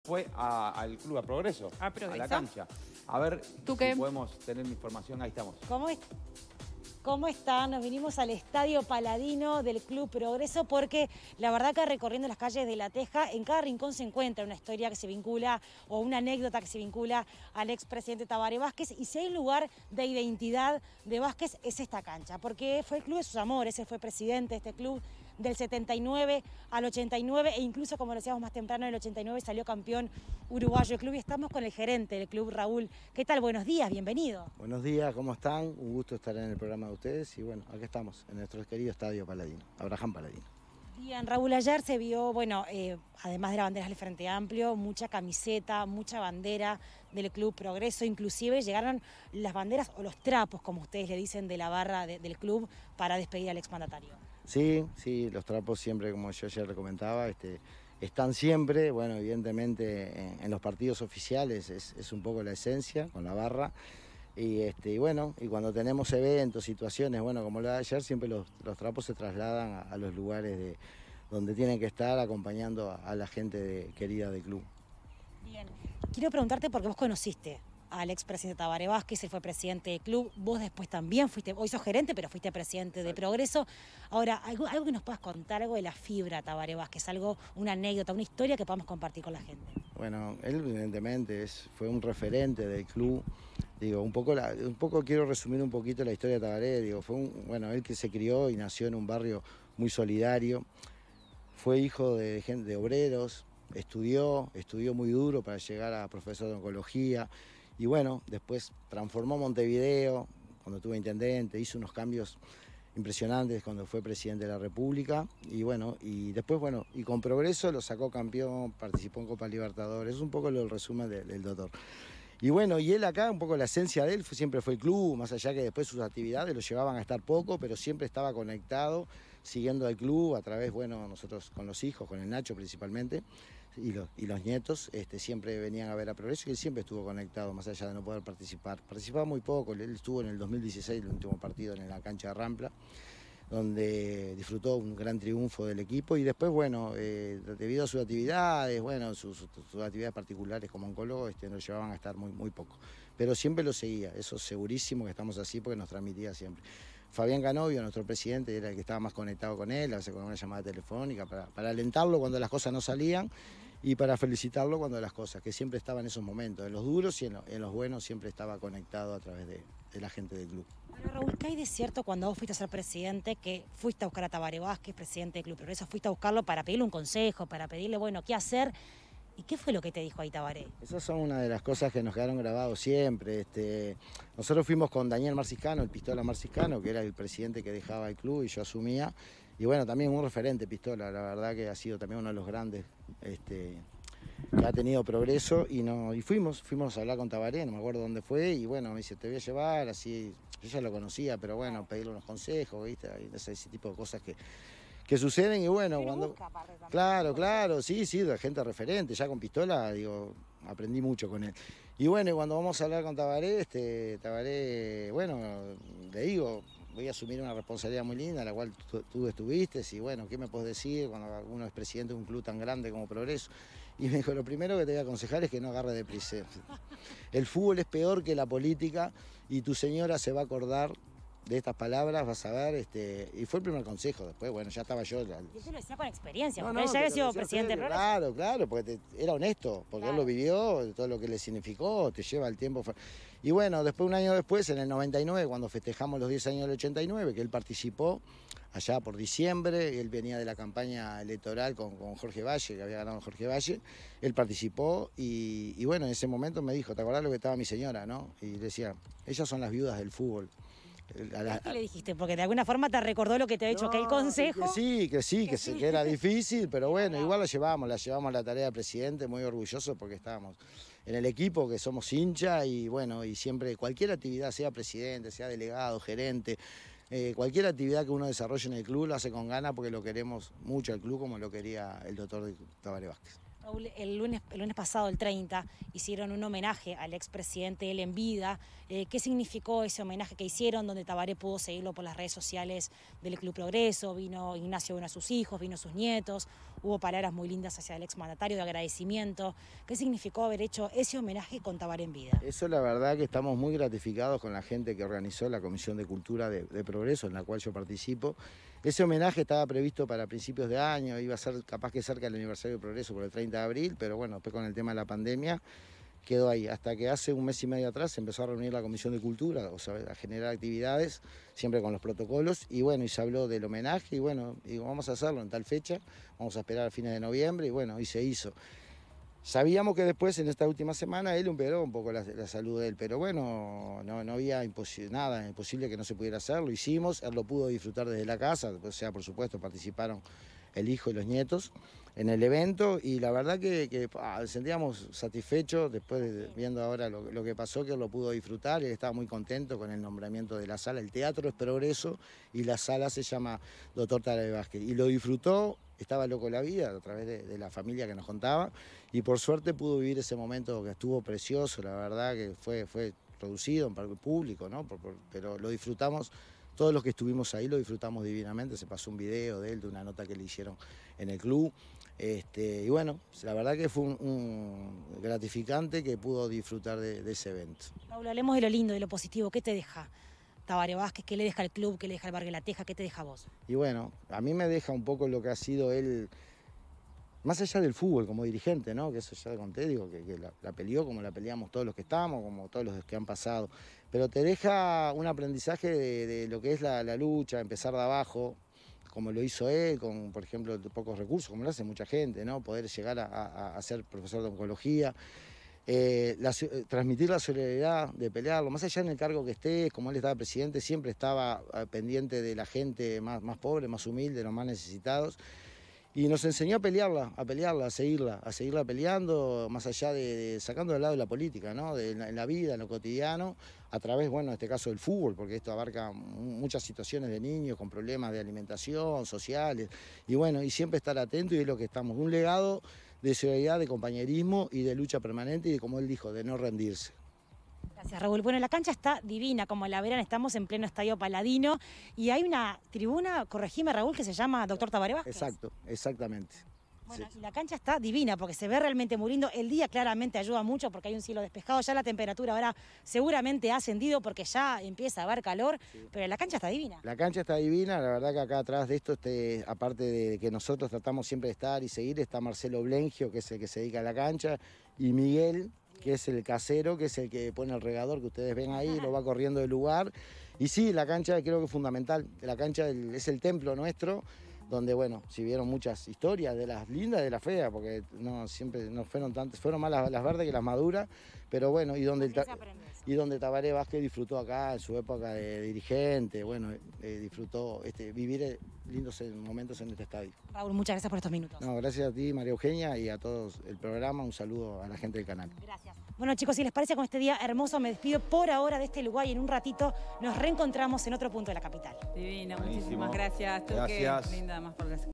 El recuerdo a Tabaré Vázquez del Club Atlético Progreso
Conversamos con el gerente del club de La Teja para conocer las anécdotas de la presencia del expresidente en la cancha de los gauchos del pantanoso  y cómo era su amor por el fútbol.